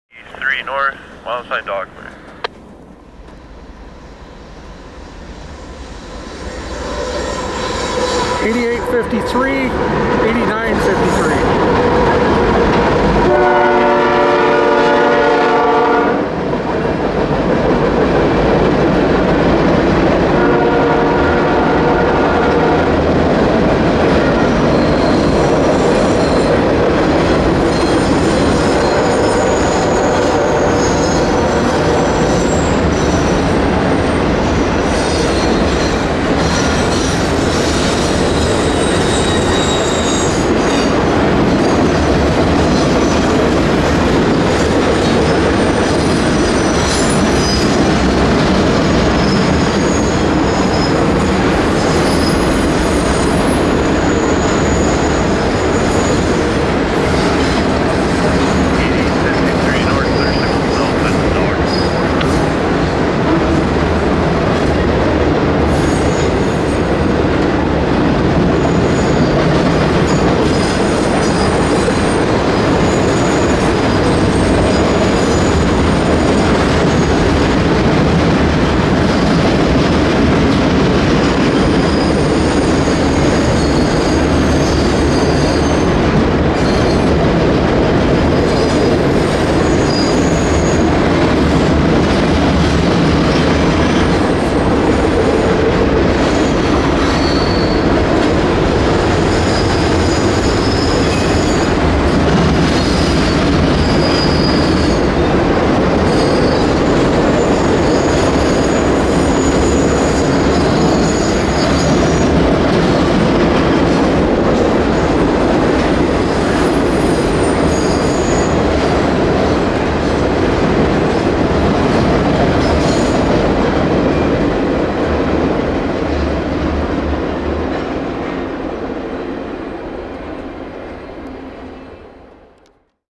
CP 8853 at Otter Lake (21NOV2015)
Canadian Pacific 8853 CP 8953 northbound passing Otter Lake on approach to Dockmure after having met CP 9641 at Brignall.